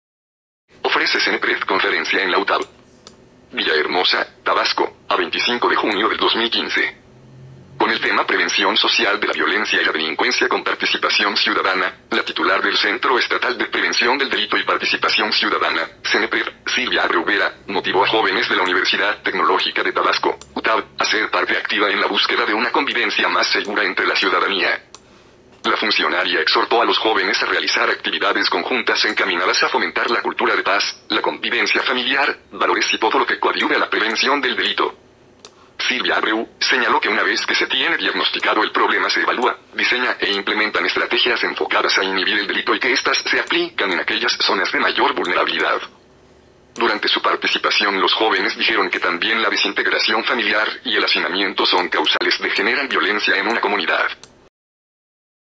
Ofrece Cenepred conferencia en la UTTAB
Villahermosa, Tabasco, a 25 de junio del 2015.

Con el tema “Prevención Social de la Violencia y la Delincuencia con participación Ciudadana”, la titular del Centro Estatal de Prevención del Delito y Participación Ciudadana (Cenepred), Silvia Abreu Vera, motivó a jóvenes de la Universidad Tecnológica de Tabasco (UTTAB) a ser parte activa en la búsqueda de una convivencia más segura entre la ciudadanía.

La funcionaria exhortó a los jóvenes a realizar actividades conjuntas encaminadas a fomentar la cultura de paz, la convivencia familiar, valores y todo lo que coadyuve a la prevención del delito.

Silvia Abreu, señaló que una vez que se tiene diagnosticado el problema se evalúa, diseña e implementan estrategias enfocadas a inhibir el delito y que estas se aplican en aquellas zonas de mayor vulnerabilidad.

Durante su participación los jóvenes dijeron que también la desintegración familiar y el hacinamiento son causales de generan violencia en una comunidad.